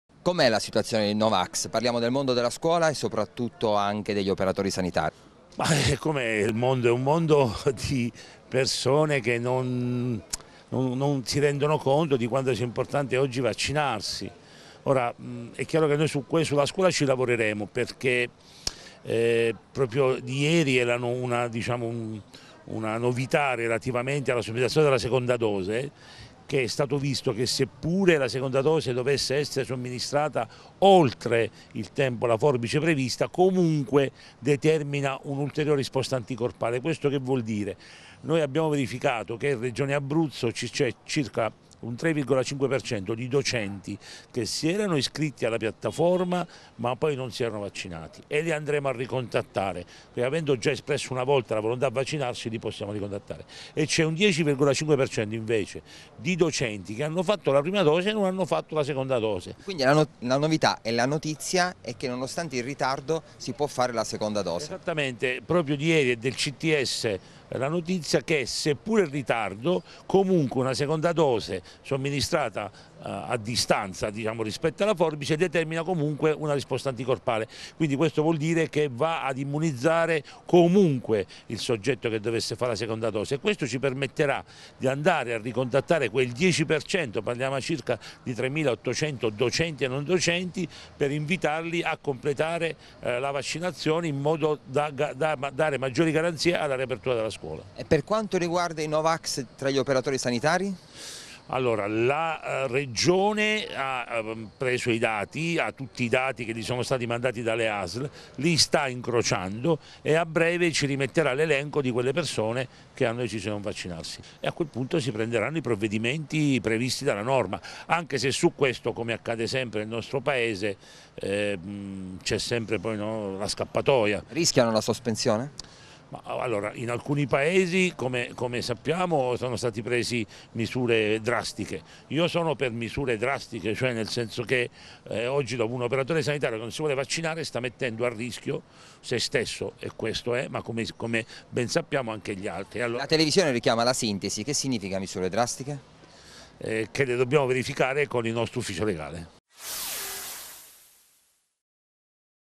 Regione Abruzzo - L'annuncio di Brucchi: "Misure drastiche per i sanitari no vax"
Il referente per i vaccini analizza la situazione